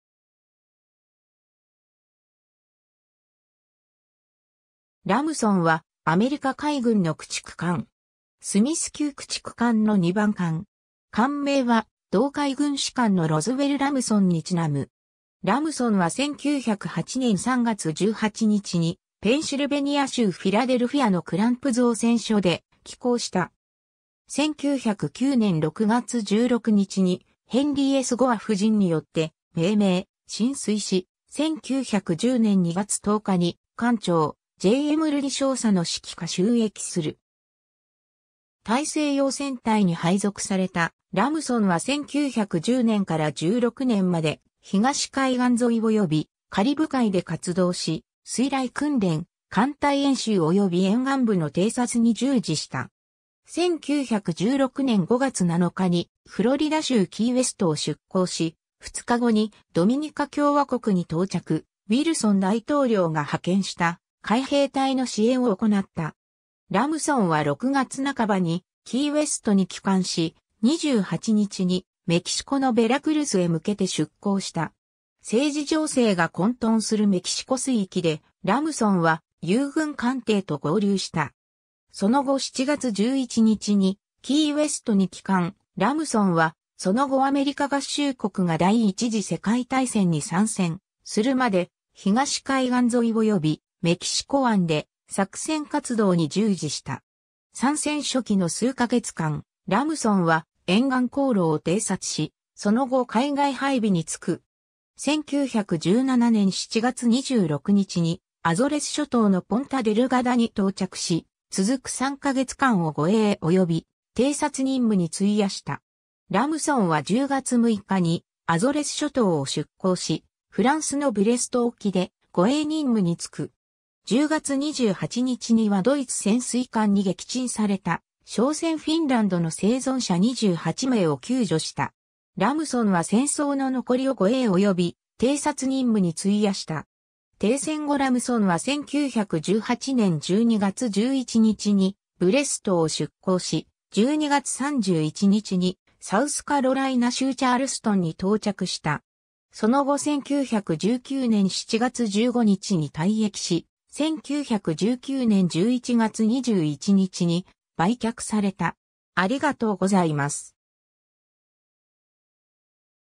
ラムソン (DD-18)
ラムソン (USS Lamson, DD-18) は、アメリカ海軍の駆逐艦。
スミス級駆逐艦の2番艦。
艦名は同海軍士官のロズウェル・ラムソンに因む。
 ラムソンは1908年3月18日にペンシルベニア州フィラデルフィアのクランプ造船所で起工した。
1909年6月16日にヘンリー・S・ゴア夫人によって命名、進水し、1910年2月10日に艦長J・M・ルディ少佐の指揮下就役する。
 大西洋戦隊に配属されたラムソンは1910年から16年まで東海岸沿いおよびカリブ海で活動し、水雷訓練、艦隊演習および沿岸部の偵察に従事した。
1916年5月7日にフロリダ州キーウェストを出航し、2日後にドミニカ共和国に到着、ウィルソン大統領が派遣した海兵隊の支援を行った。
 ラムソンは6月半ばにキーウェストに帰還し、28日にメキシコのベラクルスへ向けて出航した。
政治情勢が混沌するメキシコ水域で、ラムソンは友軍艦艇と合流した。
その後7月11日にキーウェストに帰還、ラムソンはその後アメリカ合衆国が第一次世界大戦に参戦するまで東海岸沿いおよびメキシコ湾で作戦活動に従事した。
 参戦初期の数ヶ月間、ラムソンは沿岸航路を偵察し、その後海外配備に就く。
1917年7月26日にアゾレス諸島のポンタ・デルガダに到着し、続く3ヶ月間を護衛および偵察任務に費やした。
ラムソンは10月6日にアゾレス諸島を出航しフランスのブレスト沖で護衛任務に就く。
10月28日にはドイツ潜水艦に撃沈された商船フィンランド (Finland) の生存者28名を救助した。
 ラムソンは戦争の残りを護衛および偵察任務に費やした。
停戦後ラムソンは1918年12月11日にブレストを出航し、12月31日にサウスカロライナ州チャールストンに到着した。
その後1919年7月15日に退役し、1919年11月21日に売却された。